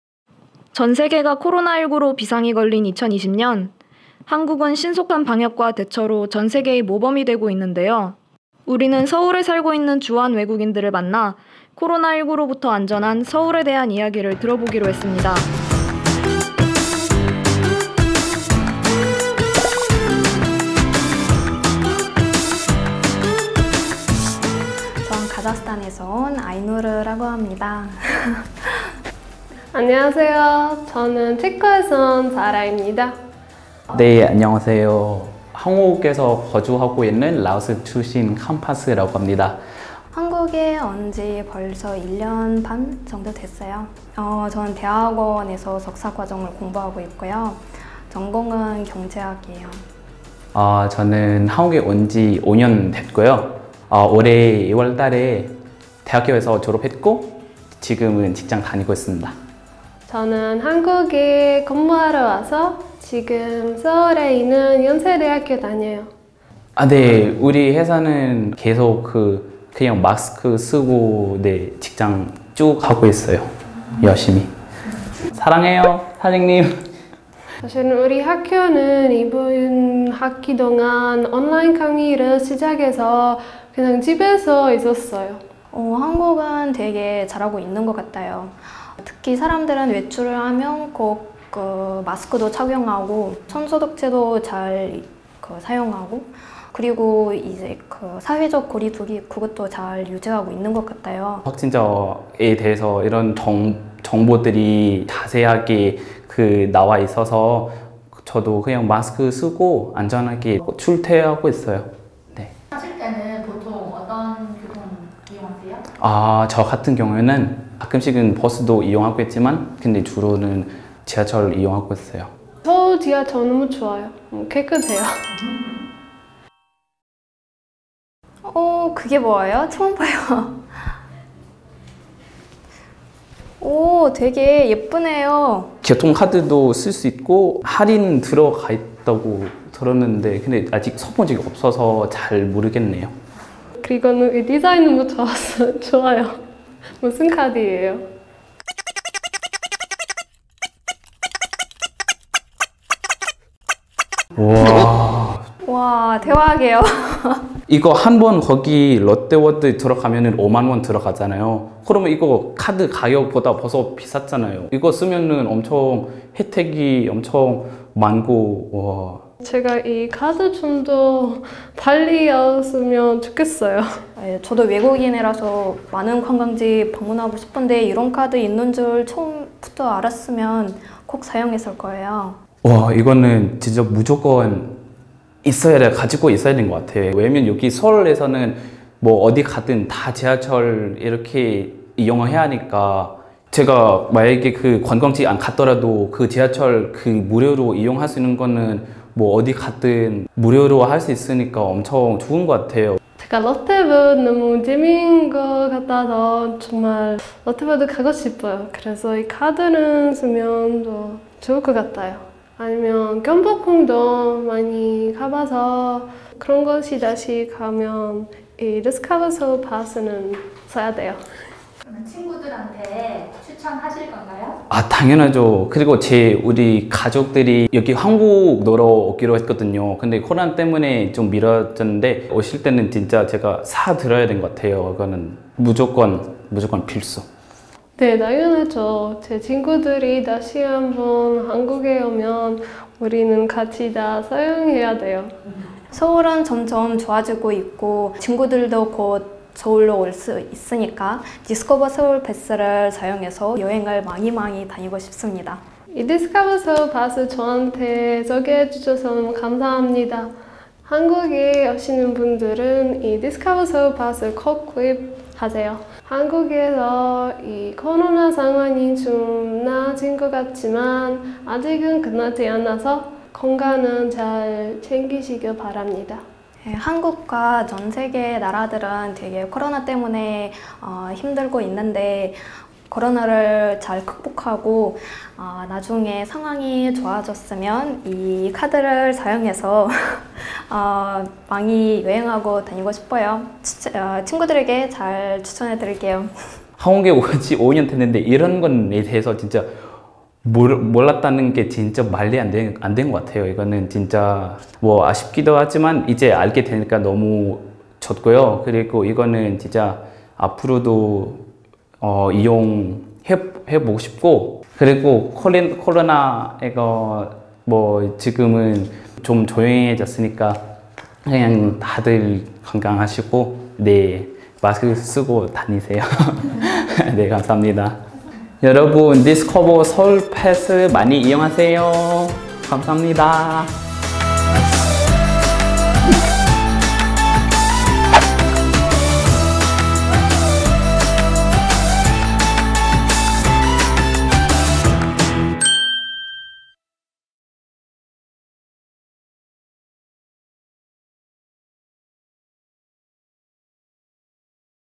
[2020 DSP] Why is Korea safe from COVID-19
How foreigners in Korea talk about COVID-19 in Seoul and what they'd like to do when this is over! Discover Seoul Pass has a story to share.